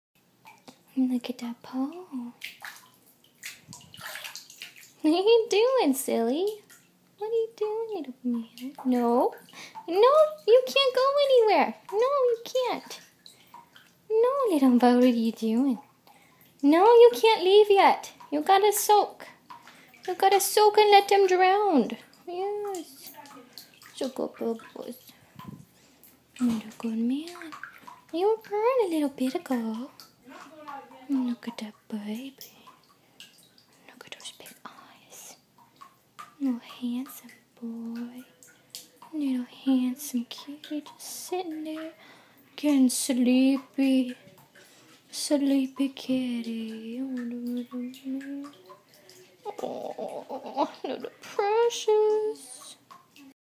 Flamepoint siamese/persian mix getting a bath.
Sorry about my dialogue; I'm a little cat obsessed.
This is my rescued cat, Porfiry. He's just over 1 year.